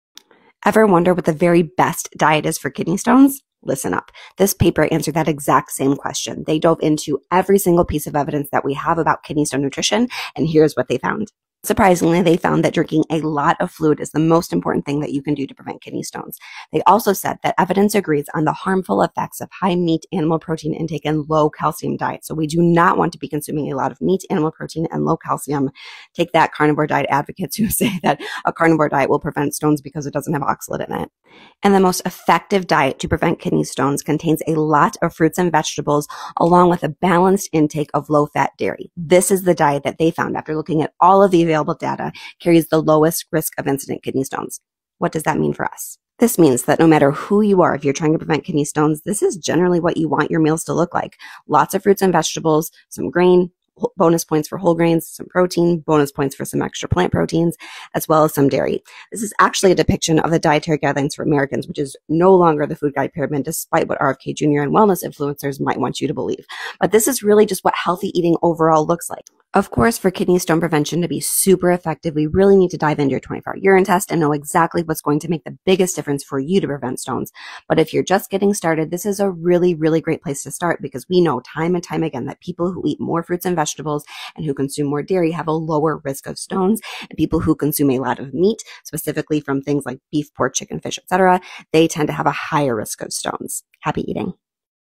The BEST Diet For Kidney Stones - Based On Actual Research
At its core, kidney stone nutrition shouldn’t be SO hard! We don’t need to be married to endless lists and be afraid of anything that has oxalate, sodium, protein or sugar in it.

To REALLY stop those stones, you just need to work with someone who has expertise in kidney stone nutrition and build healthy meal PATTERNS that include ALL foods (yes, even 🍫 if you ❤️ it!).

It isn’t so hard. I promise!

PMID: 32183500